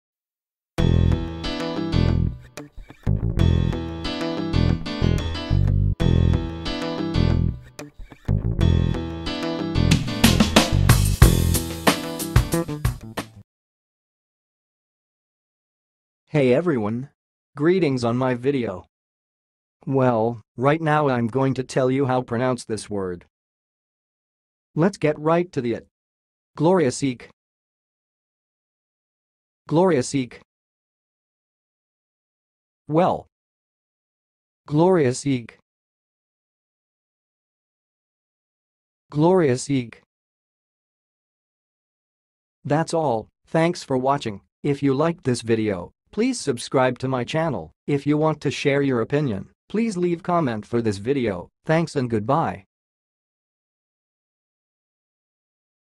GLORIOSIQUE - HOW TO PRONOUNCE IT!?
Watch how to say and pronounce "gloriosique"!